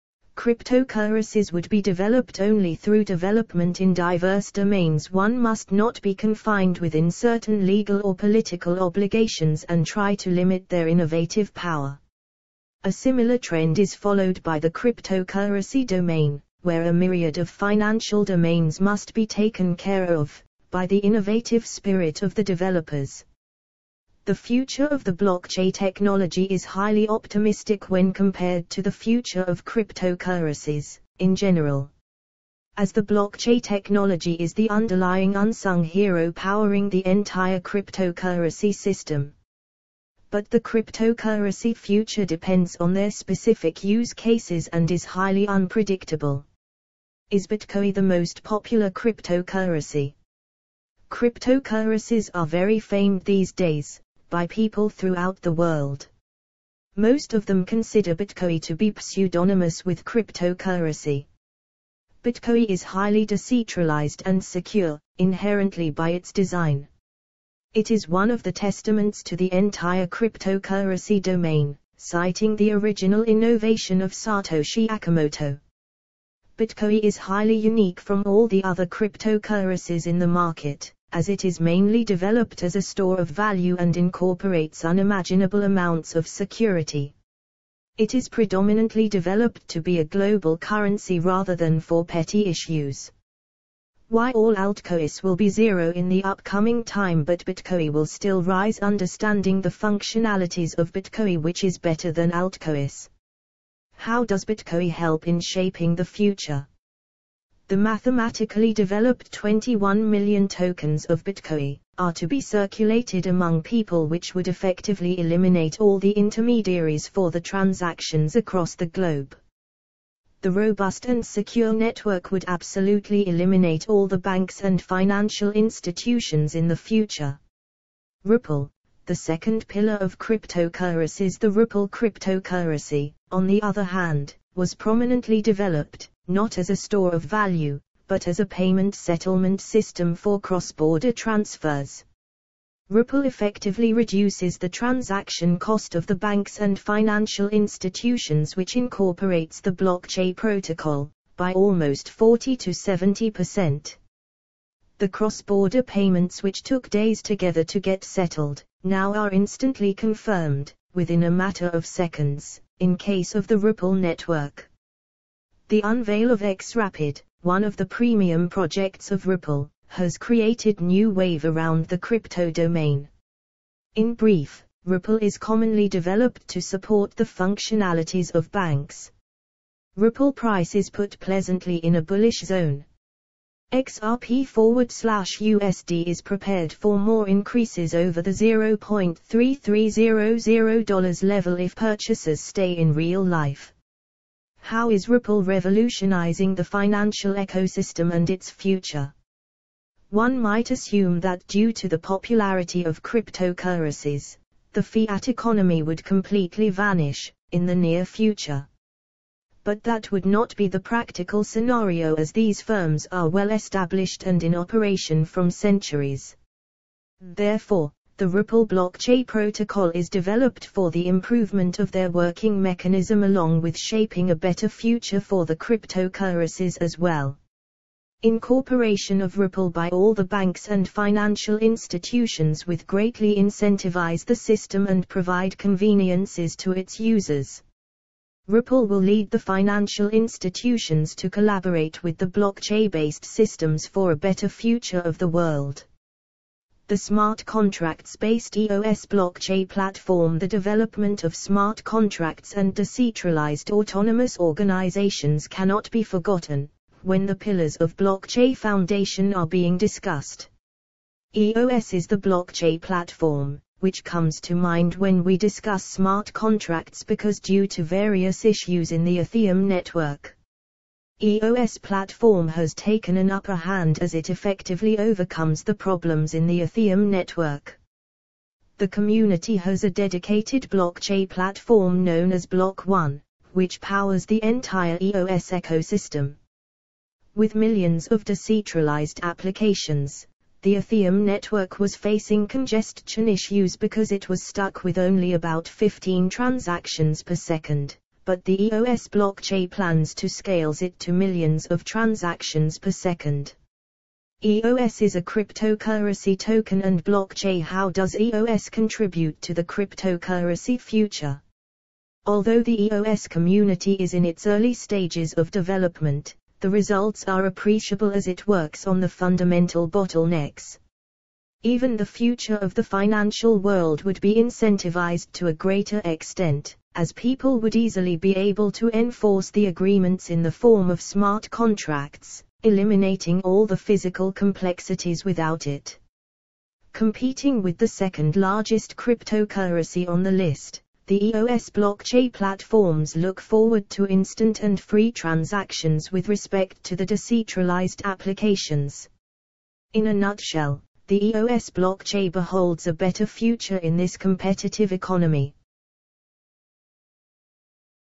Bitcoin, Ripple, EOS: The 3 pillars to Blockchain and Cryptocurrency future Ethereum is slipping?
Cryptocurrencies would be developed only through development in diverse domains
One must not be confined within certain legal or political obligations and try to limit their innovative power. A similar trend is followed by the cryptocurrency domain, where a myriad of financial domains must be taken care of, by the innovative spirit of the developers. The future of the blockchain technology is highly optimistic when compared to the future of cryptocurrencies, in general. As the Blockchain technology is the underlying unsung hero powering the entire cryptocurrency system. But the cryptocurrency future depends on their specific use cases and is highly unpredictable.

Is Bitcoin the most popular cryptocurrency?
Cryptocurrencies are very famed these days, by people throughout the world. Most of them consider Bitcoin to be pseudonymous with cryptocurrency. Bitcoin is highly decentralized and secure, inherently by its design. It is one of the testaments to the entire cryptocurrency domain, citing the original innovation of Satoshi Nakamoto. Bitcoin is highly unique from all the other cryptocurrencies in the market, as it is mainly developed as a store of value and incorporates unimaginable amounts of security. It is predominantly developed to be a global currency rather than for petty issues.

Why all altcoins will be zero in the upcoming time but bitcoin will still rise.Understanding the functionalities of Bitcoin which is better than Altcoins.

How does Bitcoin help in shaping the future?
The mathematically developed 21 million tokens of Bitcoin, are to be circulated among people which would effectively eliminate all the intermediaries for the transactions across the globe. The robust and secure network would absolutely eliminate all the banks and financial institutions in the future.

Ripple, the second pillar of cryptocurrencies
The Ripple cryptocurrency, on the other hand, was prominently developed, not as a store of value, but as a payment settlement system for cross-border transfers. Ripple effectively reduces the transaction cost of the banks and Financial Institutions which incorporates the blockchain protocol, by almost 40-70%. The cross-border payments which took days together to get settled, now are instantly confirmed, within a matter of seconds, in case of the ripple network. The unveil of xRapid, one of the premium projects of Ripple, has created new wave around the crypto domain. In brief, Ripple is commonly developed to support the functionalities of banks.

Ripple price is put pleasantly in a bullish zone. XRP/USD is prepared for more increases over the $0.3300 level if purchasers stay in real life.

How is Ripple revolutionizing the financial ecosystem and its future?
One might assume that due to the popularity of cryptocurrencies, the Fiat economy would completely vanish, in the near future. But that would not be the practical scenario as these firms are well established and in operation from centuries. Therefore, the Ripple Blockchain protocol is developed for the improvement of their working mechanism along with shaping a better future for the cryptocurrencies as well. Incorporation of Ripple by all the banks and Financial Institutions with greatly incentivize the system and provide conveniences to its users. Ripple will lead the Financial Institutions to collaborate with the blockchain-based systems for a better future of the world.

The Smart contracts based EOS blockchain platform
The development of smart contracts and decentralized autonomous organizations cannot be forgotten, when the pillars of blockchain foundation are being discussed. EOS is the Blockchain platform, which comes to mind when we discuss smart contracts because due to various issues in the Ethereum network. EOS platform has taken an upper hand as it effectively overcomes the problems in the Ethereum network. The community has a dedicated blockchain platform known as Block.one, which powers the entire EOS ecosystem. With millions of decentralized applications, the Ethereum network was facing congestion issues because it was stuck with only about 15 transactions per second, but the EOS Blockchain plans to scales it to millions of transactions per second.

EOS is a cryptocurrency token and blockchain

How does EOS contribute to the cryptocurrency future?
Although the EOS community is in its early stages of development, the results are appreciable as it works on the fundamental bottlenecks. Even the future of the financial world would be incentivized to a greater extent, as people would easily be able to enforce the agreements in the form of smart contracts, eliminating all the physical complexities without it. Competing with the second largest cryptocurrency on the list, the EOS blockchain platforms look forward to instant and free transactions with respect to the decentralized applications. In a nutshell, the EOS blockchain beholds a better future in this competitive economy.